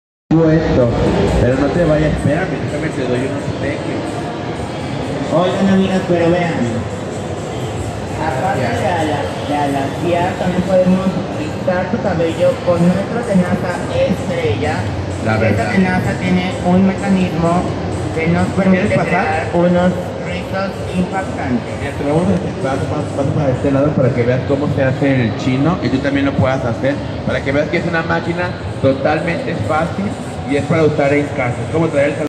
Sonorizacion con Microfonia, musicalización y conducción de evento Expo Beauty Celwh 55 1989 8574
Sonorizacion con Microfonia inalámbrica de diadema y de mano, musicalización y conducción de evento por José Jesús Torres y Miriam Cipres Bribiesca para Kiss Red Pro en la Expo Beauty Show México- fanpage oficial de Centro Banamex.